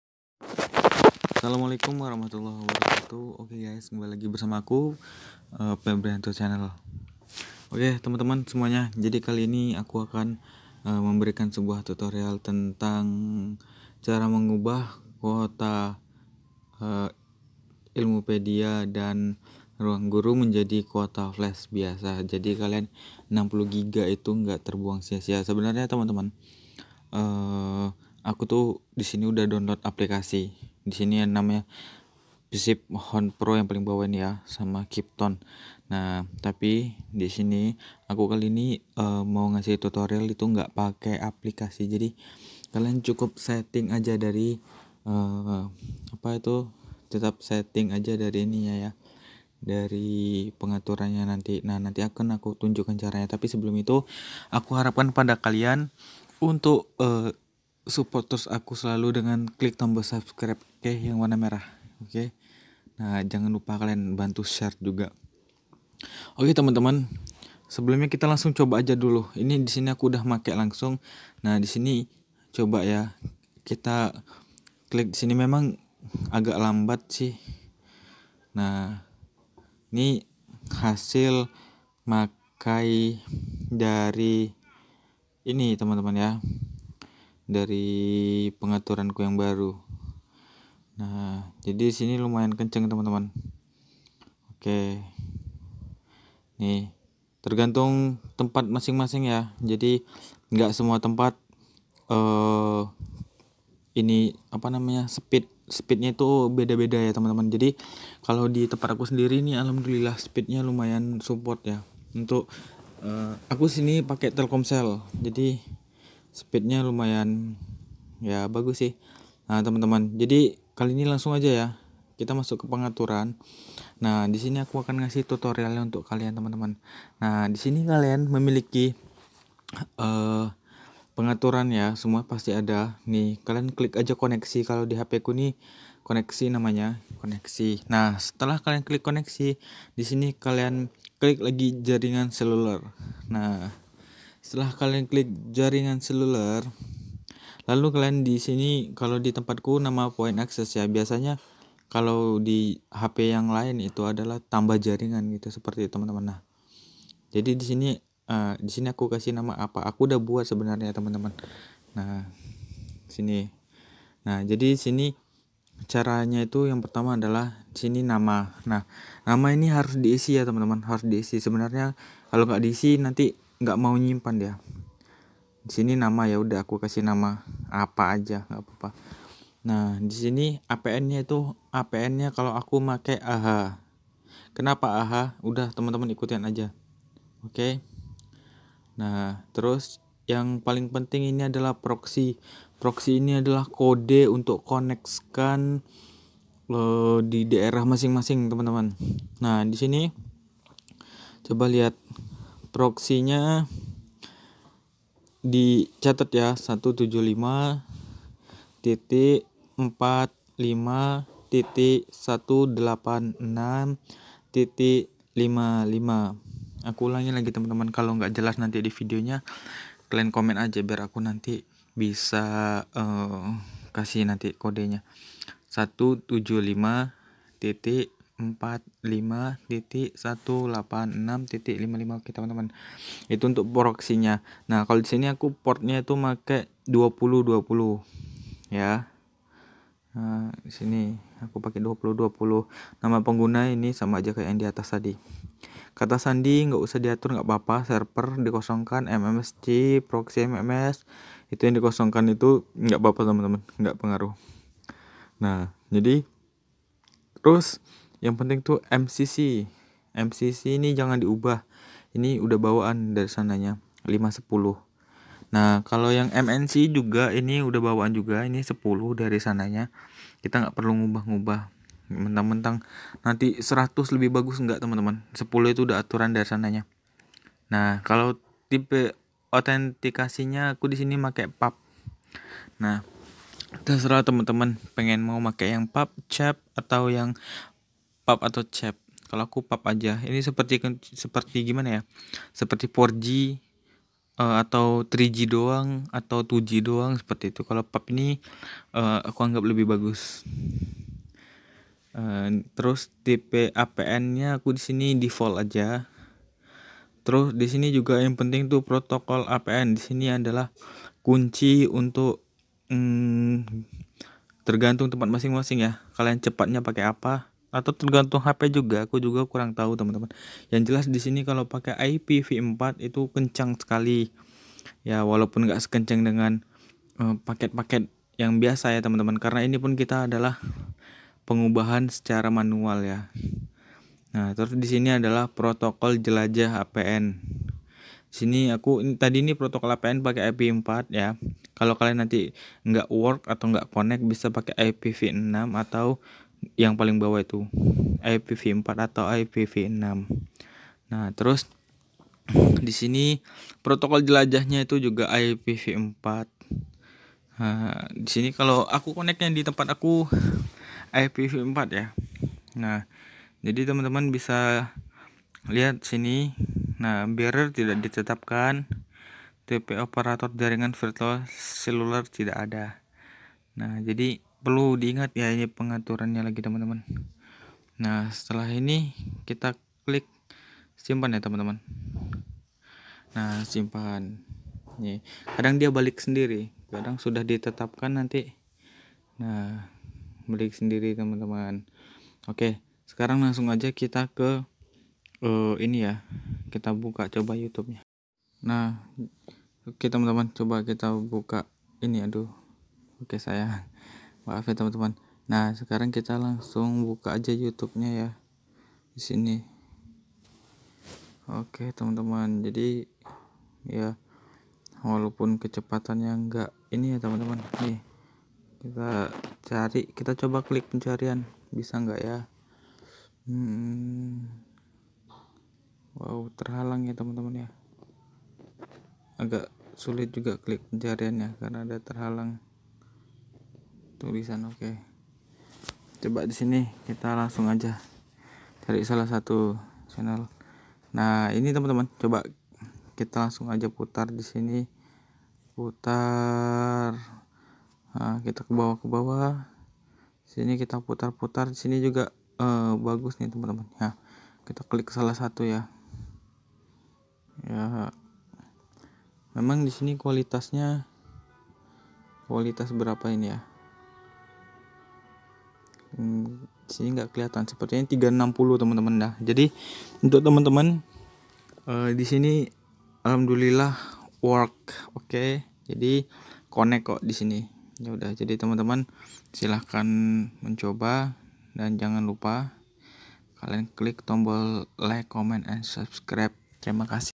3 RAHASIA PAKET SUPER MURAH 2020 - KARTU TELKOMSEL
Oke gaes jadi kali ini ak akan kasih tutorial buat kalian, semoga bermanfaat

Cara mengubah kuota RG dan IP menjadi internet flash biasa..100% no hoax

Untuk kalian coba setting  disesuaikan lokasi masing2, semoga work ya

Jangan lupa subscrinebe like komen and share